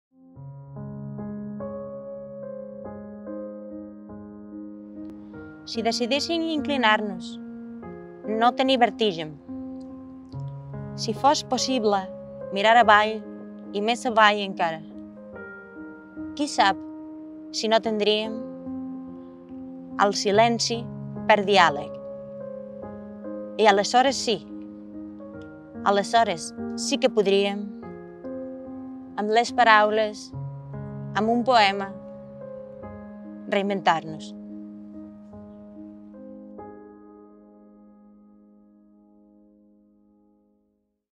"Geometria de la inclinació" de Mireia Calafell
📚 Recorregut de poesia catalana a Lisboa

A Ana, aluna de catalão na Faculdade de Letras da Universidade de Lisboa, recita o poema "Geometria de la inclinació" de Mireia Calafell no Miradouro de São Pedro de Alcântara em